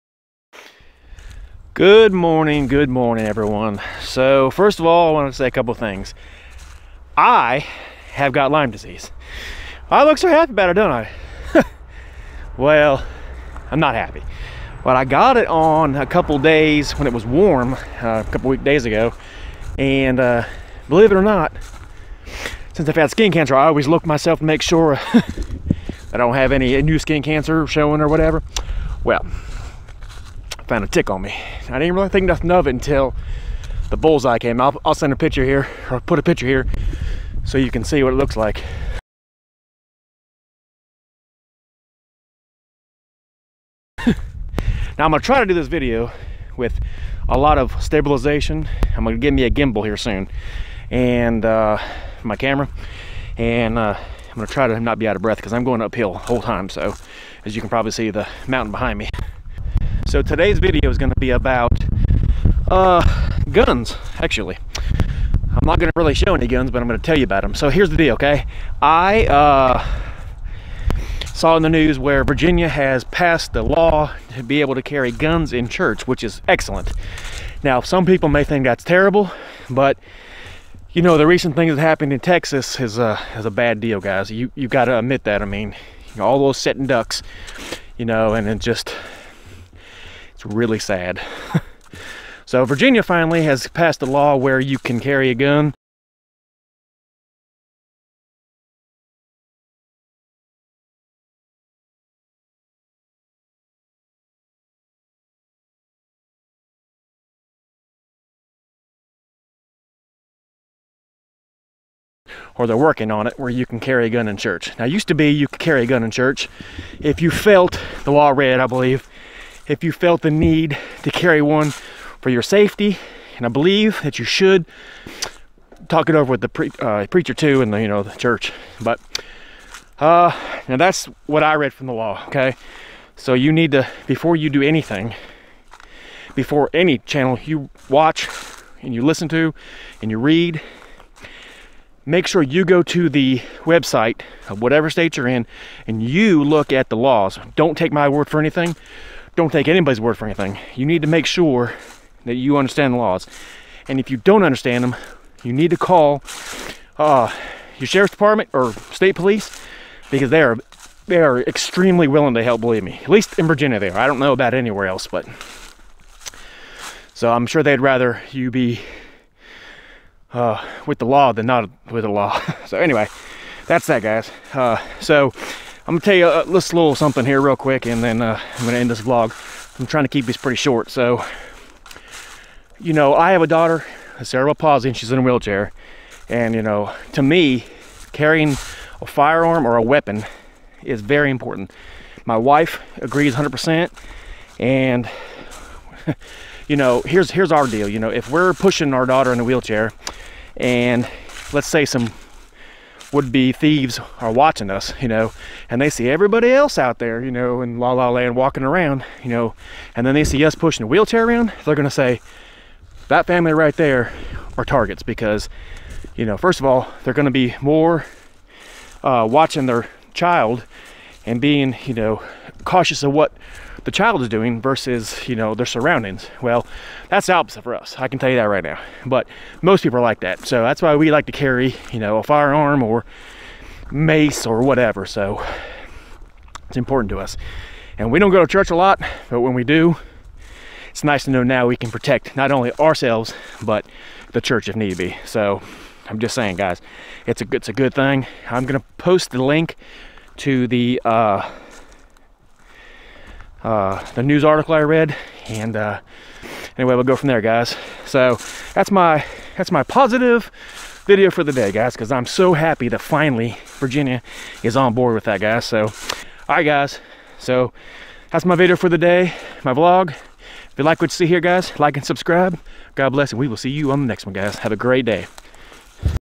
New Law For Virginia Concealed Carry In Virginia
New Law For Virginia Concealed Carry In Virginia. Its about time that Virginia gets on board with the rest of the nation.  I know this wont make a lot of people happy but it will protect alot of people and deter criminals.  Carrying concealed handgun is a must for me for my family and my protection. The laws need to keep changing for the honest law abiding citizen.